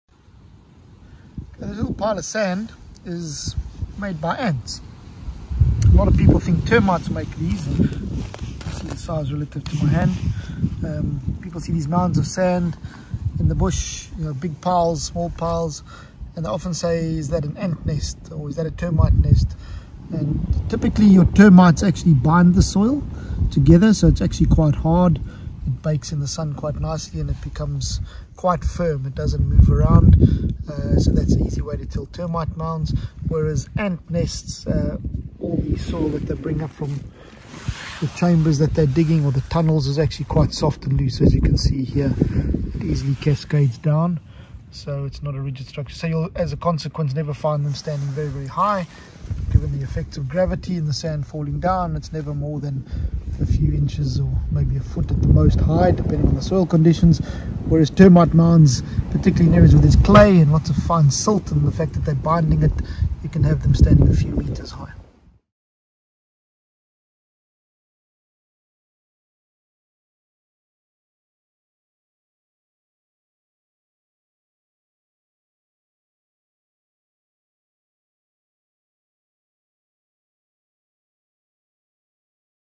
Ant Nest
Trevor explains to us how to tell an ant nest from a termite nest with a few easy points. It is important to be able to tell the difference when they are near your house or any other structure that may contain any dead wood.